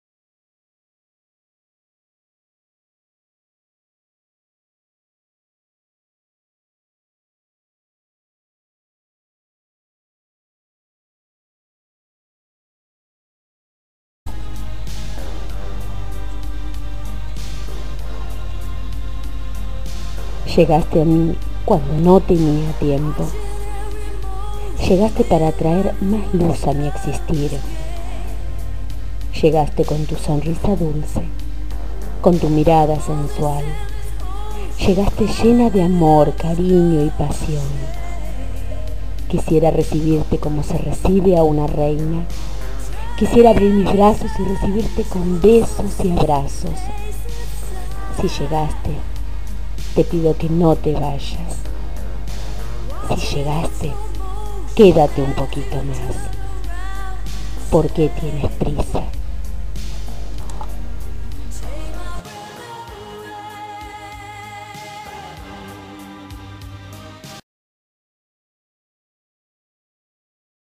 Marcial Salaverry-Poema "Llegaste a mi" voz de Liliana Varela
Un romántico poema del poeta de Brasil Marcial Salaverry en la voz de Liliana Varela